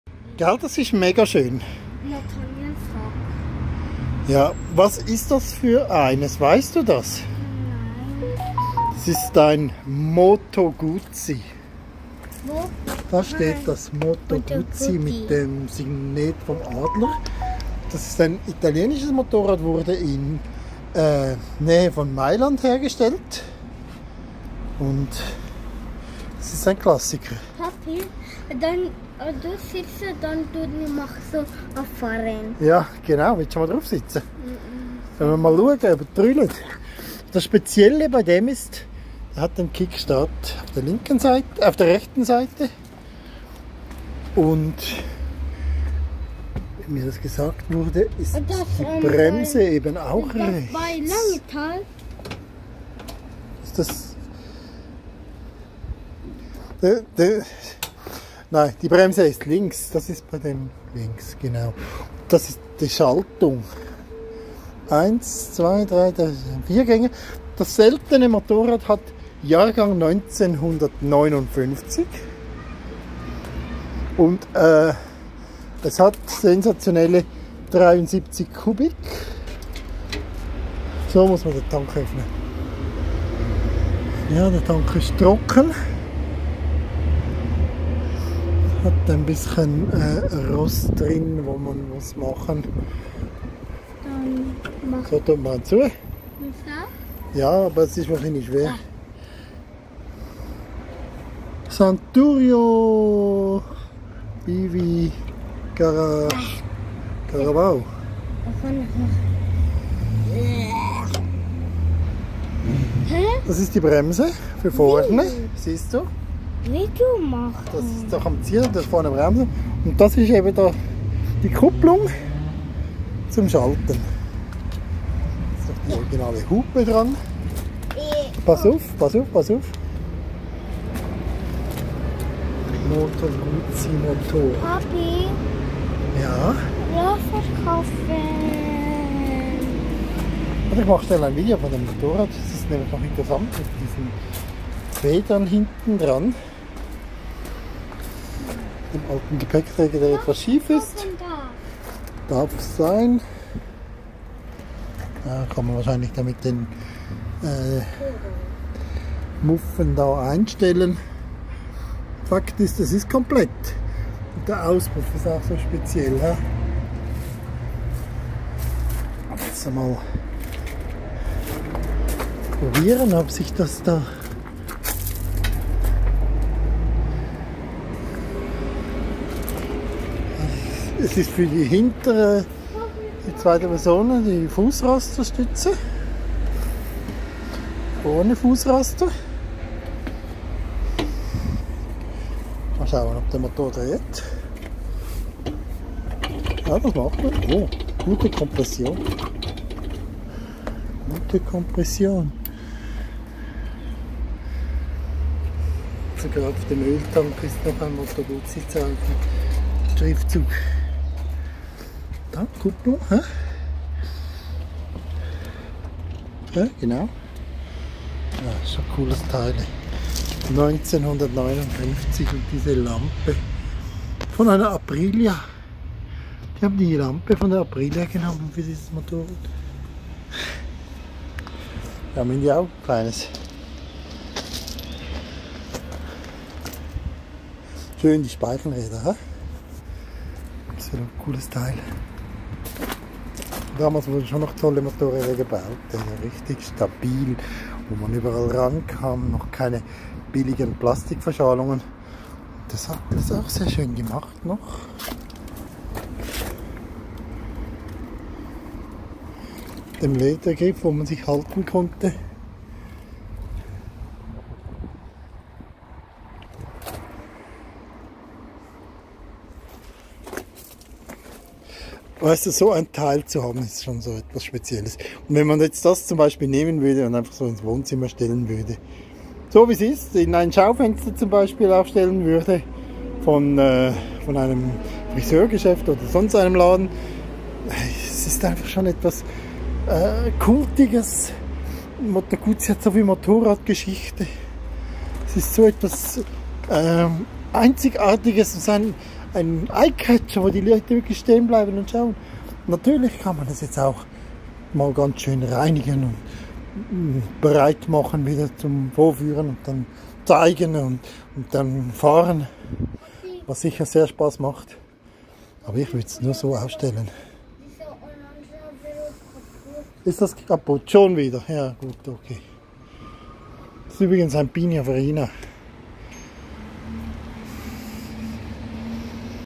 Oldtimer Moto Guzzi 1959 Cardelino
Motorrad Classic Moto Guzzi Cardelino 1959 sehr selzen, alles original Hingucker. Das wäre doch was für dich!